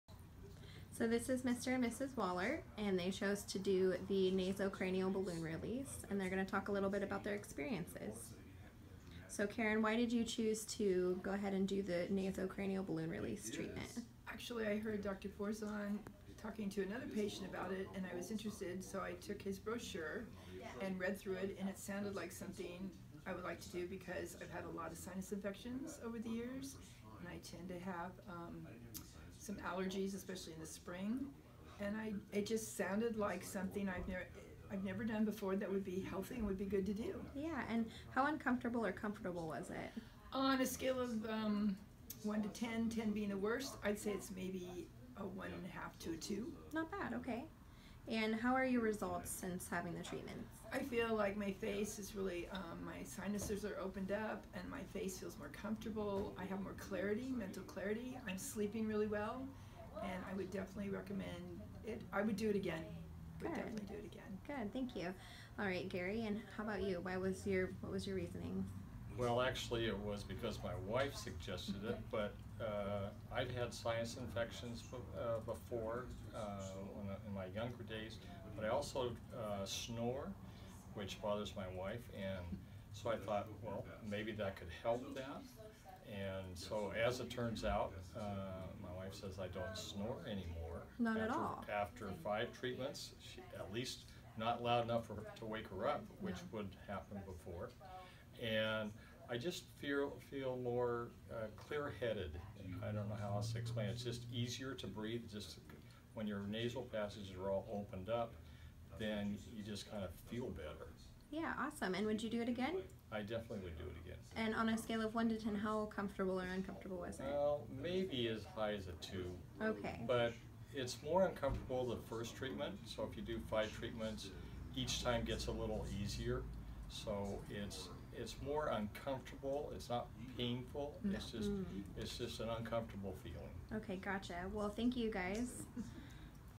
NasoCranial Balloon Release - Helped Sinus infection, Snoring & Mental Clarity
Santa Rosa and Ukiah, Ca. The NasoCranial Balloon Release is an effective technique to open up the nasal passages, breath and think better, improve cognitive function, snoring, sleep apnea and overcome concussions injuries.